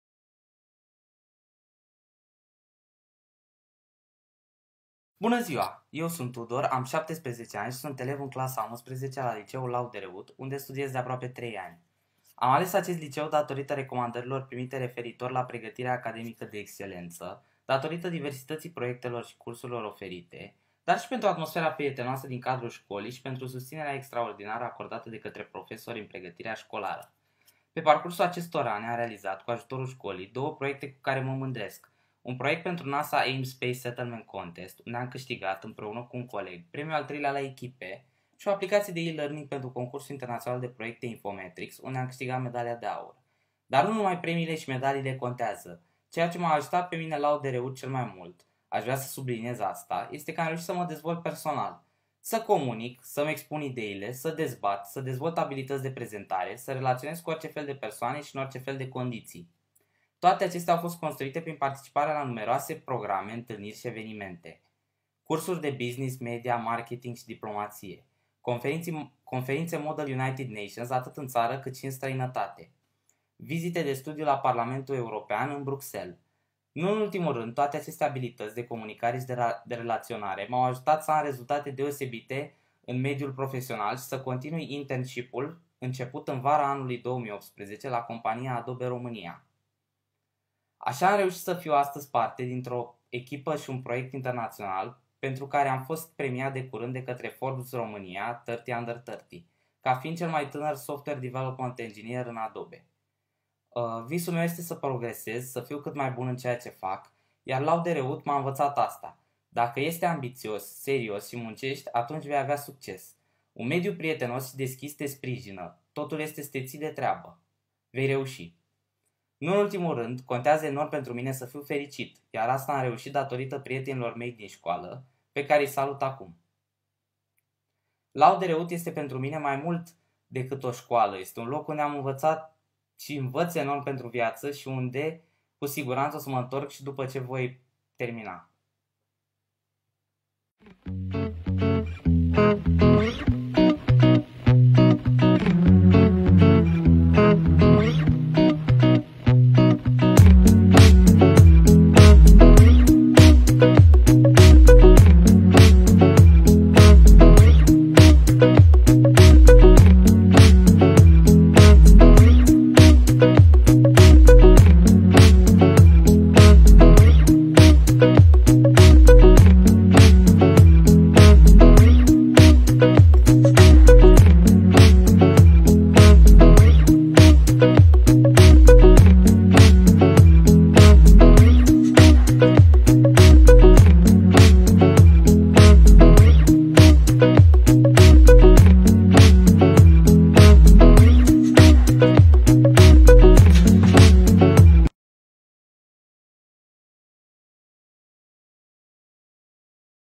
Tudor - Absolvent Laude-Reut
LAUDE-REUT LE DĂ ARIPI, EI CUCERESC LUMEA!

Este vorba de elevii și absovenții Laude-Reut, pe care vă invităm să îi cunoașteți aici, prin intermediul unei rubrici dedicată lor, cu gândurile, realizările și aspirațiile de viitori lideri ai lumii, în cariere pentru care se pregătesc cu seriozitate și determinare.

Astăzi vi-l prezentăm pe Tudor, elev in clasa a XI-a și cel mai tânăr angajat al companiei Adobe România, care ne va spune ce înseamnă Complexul Educational Laude-Reut în viața lui, de ce este importantă școala și cum educația de calitate este baza pentru devenirea personală și profesională.

Mulțumim mult, Tudor, suntem mândri de tine! Succes în continuare!